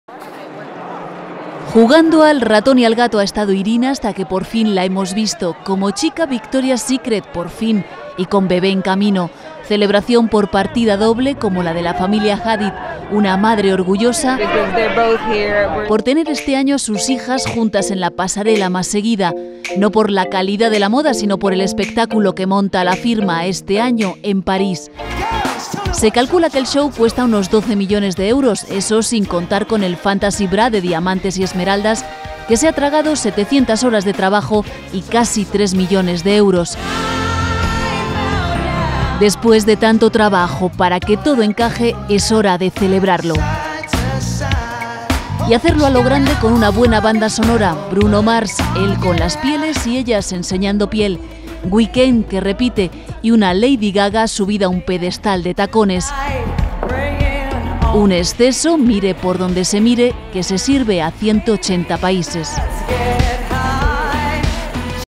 Irina Shayk desfila embarazada en Victoria's Secret.
Mi página web:
Una embarazada Irina Shayk ha participado esta noche en el desfile de Victoria's Secret. Desde hace un mes circulan rumores de que la modelo rusa está encinta fruto de su relación con el actor Bradley Cooper. Aunque no ha confirmado su estado, Shayk, una de las top en lo que se refiere a marcas de lencería, ha lucido esta noche los modelos de la firma dejando ver su nueva silueta, que evidencia que espera un bebé.